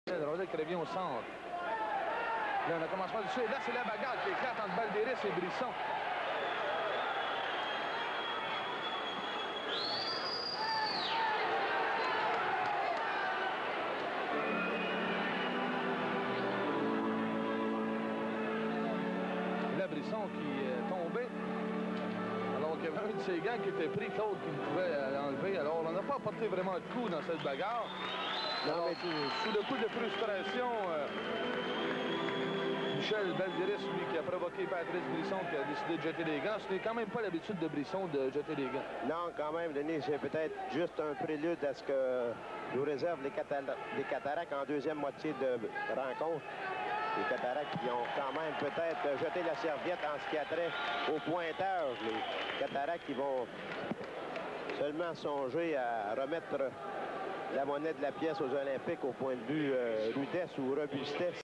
QMJHL Feb. 18, 1986 Patrice Brisson,HUL v Miguel Baldris,SHA Hull Olympiques Shawinigan Cataractes
QMJHL February 18, 1986 Patrice Brisson,HUL v Miguel Baldris,SHA